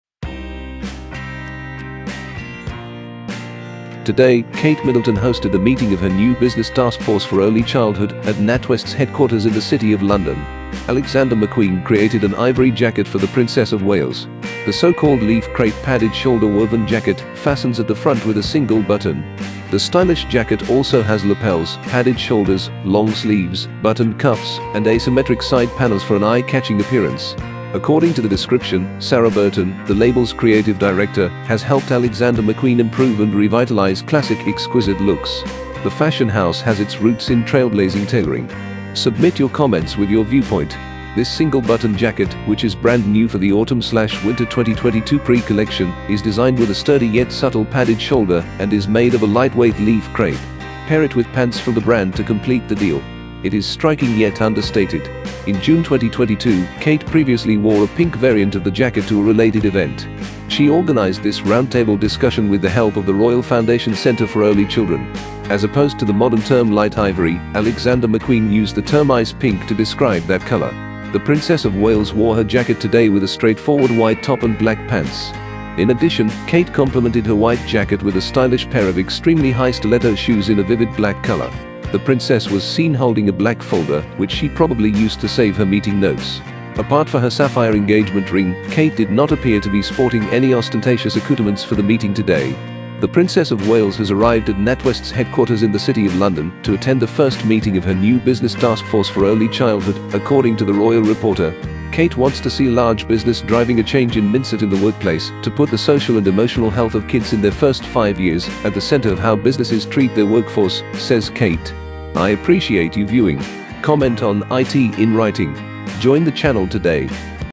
ATTENTION! Princess Catherine wore a colossal jacket in London to meet her new business group
Princess Catherine wore a colossal jacket in London to meet her new business group

Today, Kate Middleton hosted a meeting of her new Business Taskforce for Early Childhood at NatWest's headquarters in the City of London. Alexander McQueen created an ivory jacket for the Princess of Wales.
The so-called "leaf crepe padded-shoulder woven jacket" fastens at the front with a single button.
The stylish jacket also has lapels, padded shoulders, long sleeves, buttoned cuffs, and asymmetric side panels for an eye-catching appearance.
According to the description, Sarah Burton, the label's creative director, has helped Alexander McQueen improve and revitalize classic, exquisite looks. The fashion house has its roots in trailblazing tailoring.
SUBMIT YOUR COMMENTS WITH YOUR VIEWPOINT!
This single-button jacket, which is brand-new for the Autumn/Winter 2022 pre-collection, is designed with a sturdy yet subtle padded shoulder and is made of a lightweight leaf crepe. Pair it with pants from the brand to complete the deal. It is striking yet understated.
In June 2022, Kate previously wore a pink variant of the jacket to a related event.
She organized this roundtable discussion with the help of the Royal Foundation Centre for Early Children.
As opposed to the modern term "light ivory," Alexander McQueen used the term "ice pink" to describe that color.
The Princess of Wales wore her jacket today with a straightforward white top and black pants.
In addition, Kate complemented her white jacket with a stylish pair of extremely high stiletto shoes in a vivid black color.
The Princess was seen holding a black folder, which she probably used to save her meeting notes.
Apart for her sapphire engagement ring, Kate did not appear to be sporting any ostentatious accoutrements for the meeting today.
The Princess of Wales has arrived at NatWest's headquarters in the City of London to attend the first meeting of her new Business Taskforce for Early Childhood, according to the royal reporter.
"Kate wants to see large business driving a change in mindset in the workplace to put the social and emotional health of kids in their first five years at the center of how businesses treat their workforce," says Kate.
I APPRECIATE YOU VIEWING!
COMMENT ON IT IN WRITING!